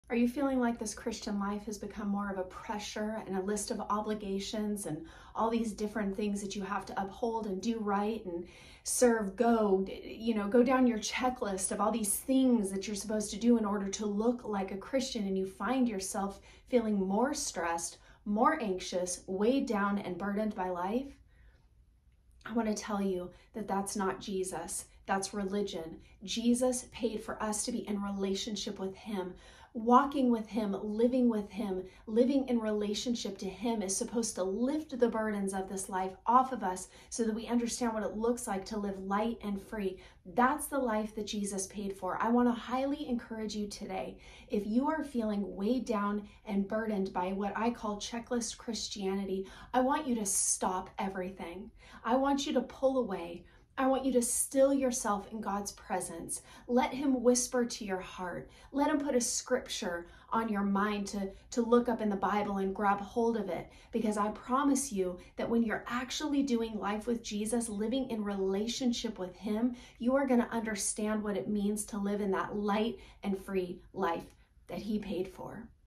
Are you a Christian but stressed & overwhelmed?
Somewhere, somehow, we have taken what was meant to be a restored relationship to a list of duties and obligations that lead us to burn out. To my church family, this is not God’s heart for any of us. Life with God should lift the burdens off not make us feel stretched thin and weighed down by life. Amen? If this video is speaking to you, you’d love my book Ripple Effect.

Amazon Ripple Effect: A Transformational Journey into God's Heart That Will Change You from the Inside Out